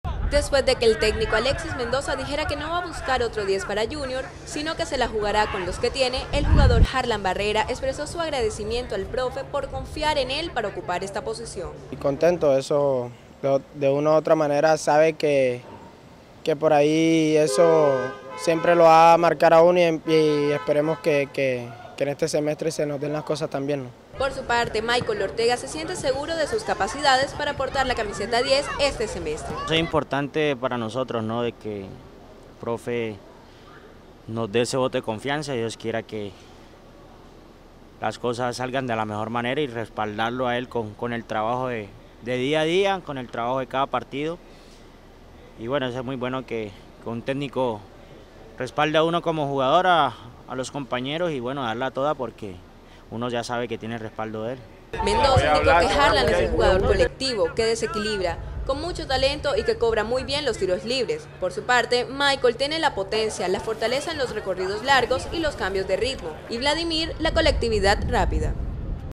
Junior se la jugará con Ortega, Hernández y Jarlan Barrera
Los jugadores rojiblancos Michael Ortega, Vladimir Hernández y Jarlan Barrera serán los encargados de tomar las riendas creativas del plantel rojiblanco, tras la salida de Macnelly Torres. Escucha aquí sus declaraciones.

¡Únete a nuestra comunidad virtual!